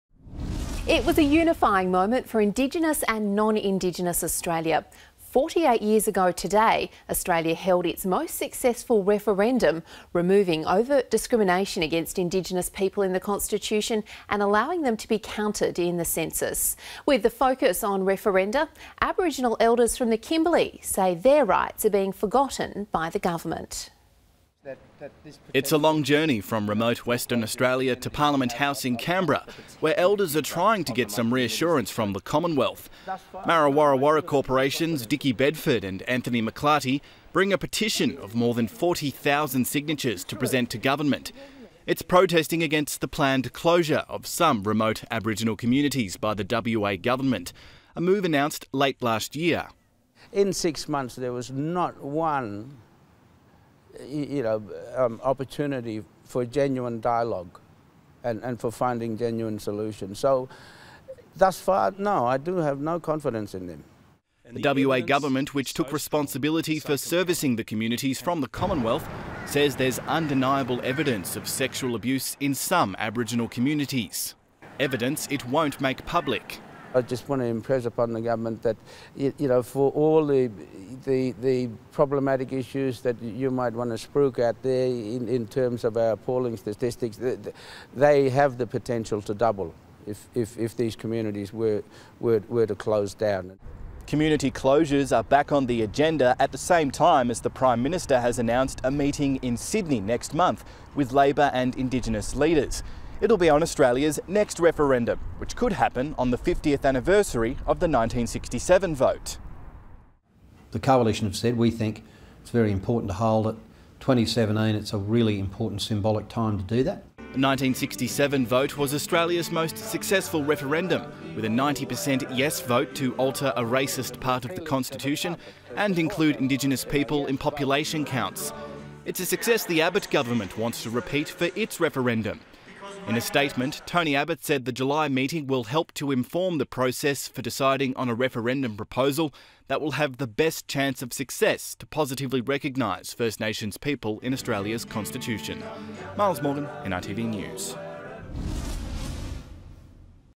WA Elders concerned government may 'forget' their rights on referendum anniversary
It was a unifying moment for Indigenous and non-Indigenous Australia. 48 years ago today, Australia held its most successful referendum  - removing overt discrimination against Indigenous people in the Constitution and allowing them to be counted in the census. With the focus on referenda, Aboriginal elders from the Kimberley say their rights are being forgotten by the Government.